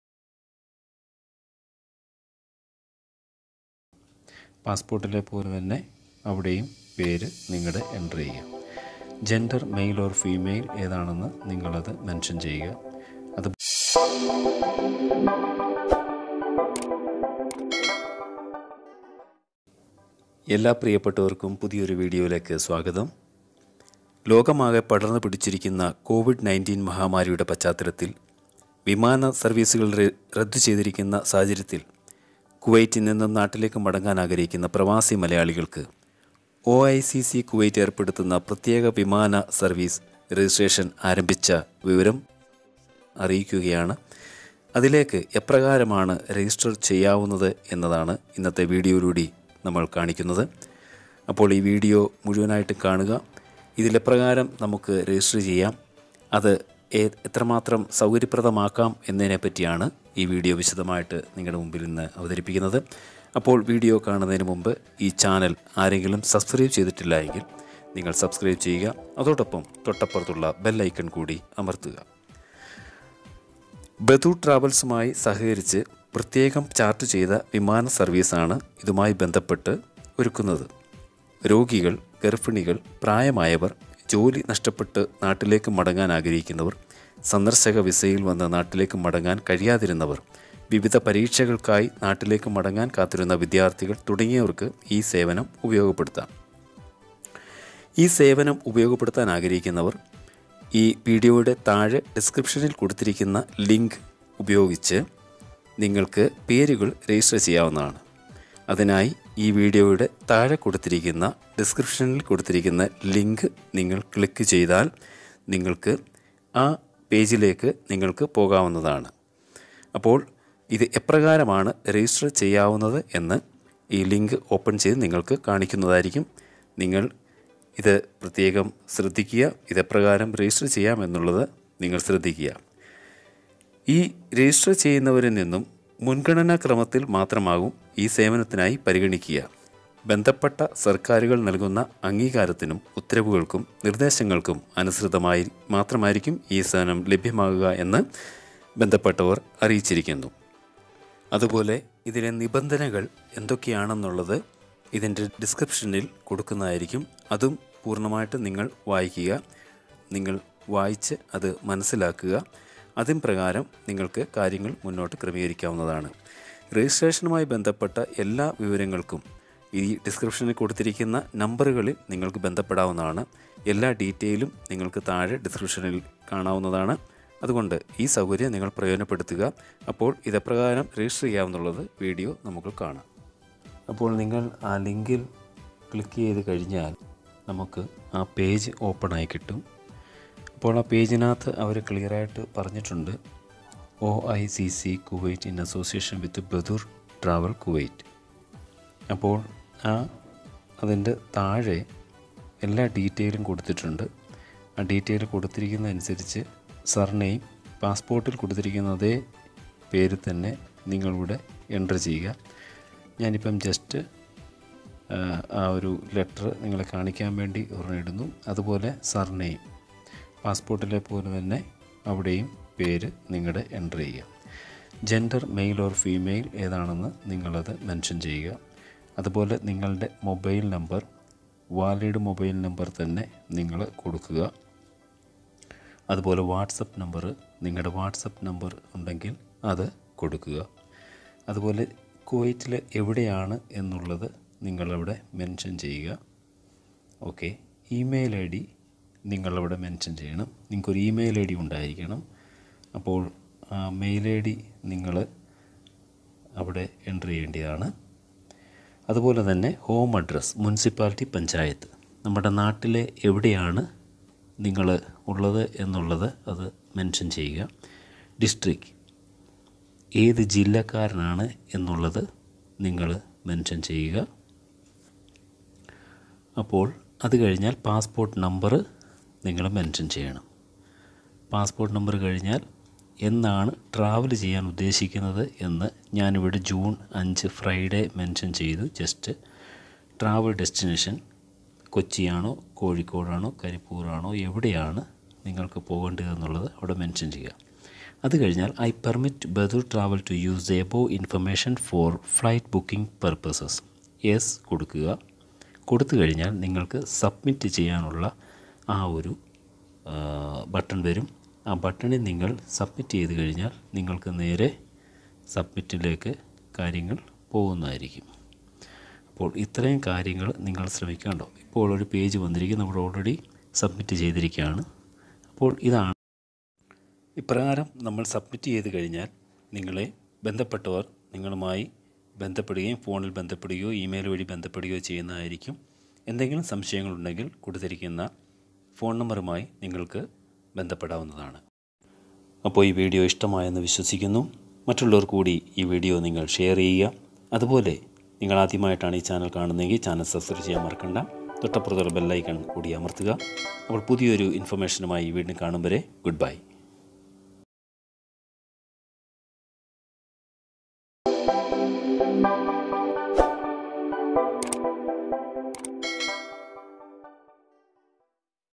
How to Register Charted flights to India|| നാട്ടിലേക്ക് പോകുവാൻ വിമാന ചാർട്ട് എങ്ങനെ Register ചെയ്യാ
Registration formalities for Charted flights to India
ഒഐസിസി കുവൈറ്റ്‌ ചാർട്ടേഡ്‌ വിമാന സർവ്വീസ്‌ രജിസ്ട്രേഷൻ ആരംഭിച്ചു.

എന്ന ലിങ്കിൽ ക്ലിക്ക് ചെയ്ത് പേരുകൾ രജിസ്റ്റർ ചെയ്യേണ്ടതാണ്. രജിസ്റ്റർ ചെയ്യുന്നവരിൽ നിന്നും മുൻ‌ഗണന ക്രമത്തിൽ മാത്രമാവും ഈ സേവനത്തിനായി പരിഗണിക്കുക. ബന്ധപ്പെട്ട സർക്കാരുകൾ നൽകുന്ന അംഗീകാരത്തിനും, ഉത്തരവുകൾക്കും, നിർദ്ദേശങ്ങൾക്കും അനുസൃതമായി മാത്രമായിരിക്കും ഈ സേവനം ലഭ്യമാവുക. പേരുകൾ രജിസ്റ്റർ ചെയ്യുന്നവർ താഴെ പറയുന്ന വ്യവസ്ഥകൾ പാലിക്കപ്പെട്ടിട്ടുണ്ട്/പാലിക്കപ്പെടും എന്ന് ഉറപ്പു വരുത്തേണ്ടതാണ്.

1. യാത്ര ചെയ്യുന്നതിന്റെ ചിലവുകൾ പൂർണ്ണമായും യാത്രക്കാർ തന്നെ വഹിക്കേണ്ടതാണ്. ഏതെങ്കിലും കാരണങ്ങളാൽ യാത്ര മുടങ്ങിയാൽ ഈ ചിലവുകൾ റീഫണ്ട് ചെയ്യുന്നതല്ല.

2. കുവൈറ്റിലെ നിയമാനുസൃതമായ താമസക്കാർക്കും, ലോക്ക്ഡൌൺ കാലഘട്ടത്തിൽ വിസാ നിയമങ്ങളിൽ ഇളവുകൾ ലഭിച്ചവർക്കും മാത്രമാണ് ഈ സേവനം ഉപയോഗിക്കുവാൻ കഴിയുക.

3. യാത്ര ചെയ്യാനാഗ്രഹിക്കുന്നവർ വിമാനത്തിൽ പ്രവേശിക്കുന്നതിനു മുമ്പായി നാട്ടിലെത്തിയാൽ ഇന്ത്യ ഗവൺ‌മെന്റും കേരള ഗവൺ‌മെന്റും ഏർപ്പെടുത്തിയിട്ടുള്ള കോവിഡ് 19 പ്രോട്ടോക്കോളും/നിബന്ധനകളും പാലിച്ചു കൊള്ളാമെന്ന സത്യവാങ്ങ്മൂലം നൽകേണ്ടതാണ്.

4. വിമാനത്തിൽ പ്രവേശിക്കുന്നതിനു മുമ്പായി നടത്തുന്ന പരിശോധനകളിൽ ഏതെങ്കിലും തരത്തിൽ രോഗലക്ഷണങ്ങൾ പ്രകടിപ്പിക്കുന്നവരെ യാത്ര ചെയ്യാൻ അനുവദിക്കുന്നതല്ല.

5. യാത്ര ചെയ്യുന്നവർ അവരുടെ മൊബൈൽ ഫോണുകളിൽ കേന്ദ്ര ഗവൺ‌മെന്റിന്റെ ആരോഗ്യസേതു ആപ്ലിക്കേഷൻ ഇൻസ്റ്റാൾ ചെയ്യേണ്ടതാണ്.

6. യാത്ര ചെയ്യുന്നവർ എല്ലാവരും എയർപോർട്ടിൽ ആരോഗ്യ വകുപ്പ് നിർദ്ദേശിച്ചിട്ടുള്ള സാക്ഷ്യപത്രം പൂരിപ്പിച്ച് ആരോഗ്യ/എമിഗ്രേഷൻ വിഭാഗങ്ങളെ ഏൽപ്പിക്കേണ്ടതാണ്.

രജിസ്ട്രേഷനുമായ്‌ ബന്ധപ്പെട്ട വിവരങ്ങൾക്ക് താഴെ പറയുന്ന നമ്പറുകളിൽ ബന്ധപ്പെടുക.
60617882, 97855354, 99776185, 51063111